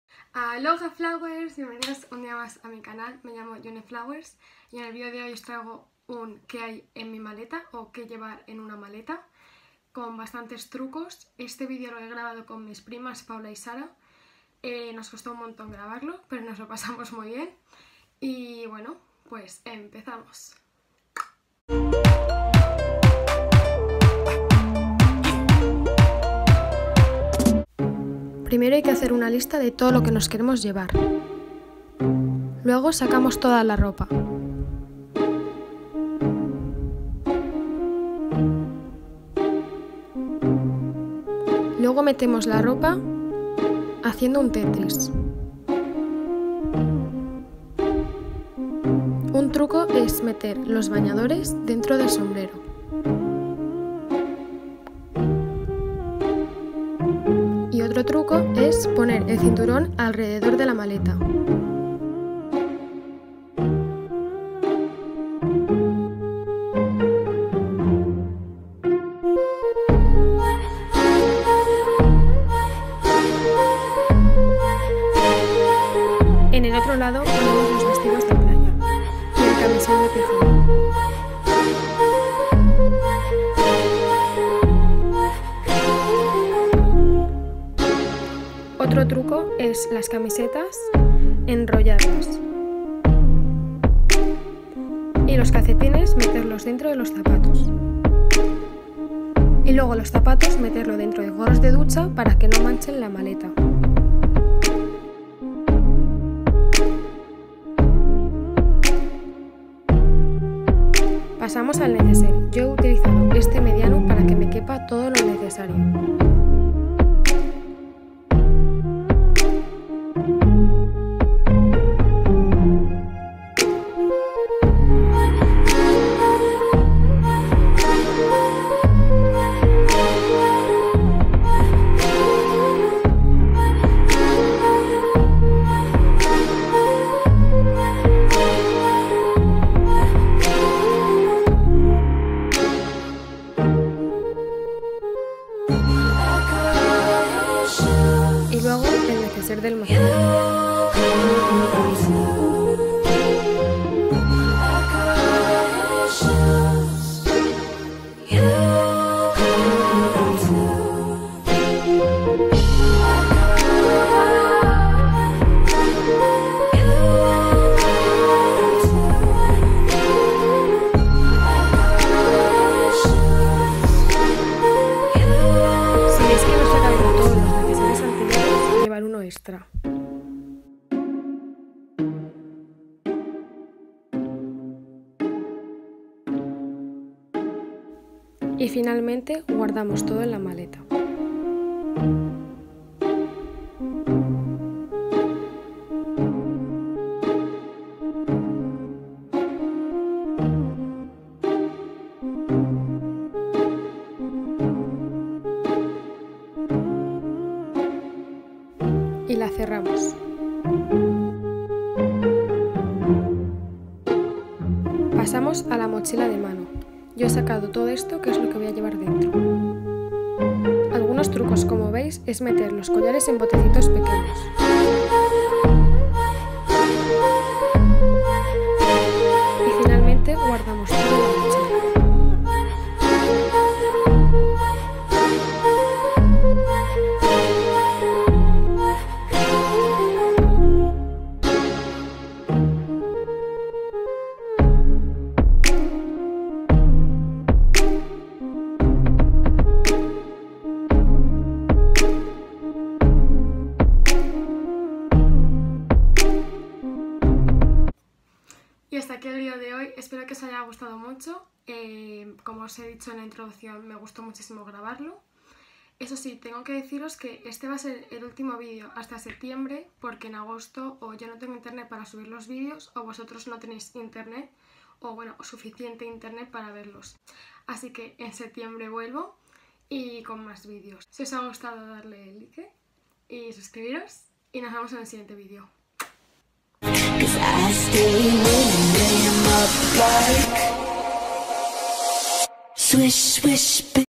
QUE HAY EN MI MALETA?
ALOHA FLOWEERS! Hoy os traigo el que hay en mi maleta y es el ultimo video hasta septiembre.